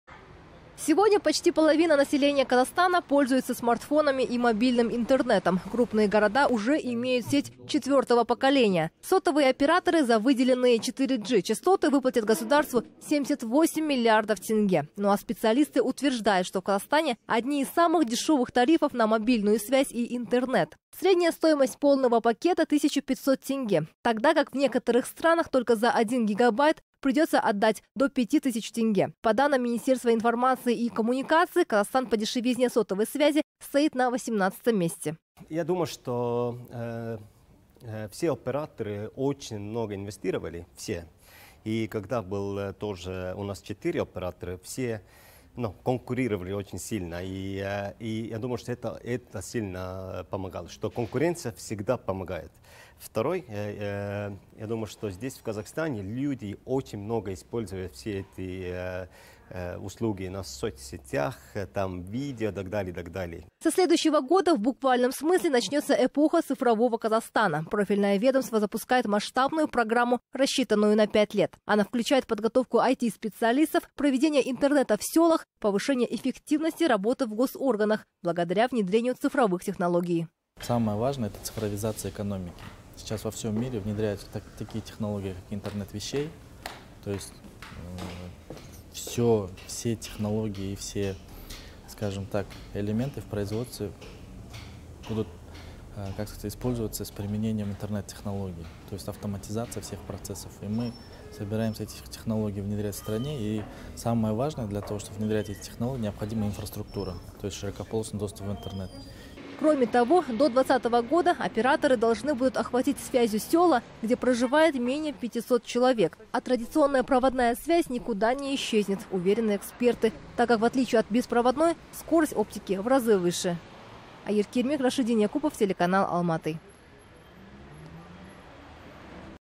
С 2017 года начнется эпоха цифрового Казахстана (31.08.16)
Со следующего года в буквальном смысле начнется эпоха цифрового Казахстана. Профильное ведомство запускает масштабную программу, рассчитанную на 5 лет. Она включает подготовку ай-ти специалистов, проведение интернета в селах, повышение эффективности работы в госорганах  благодаря внедрению цифровых технологий.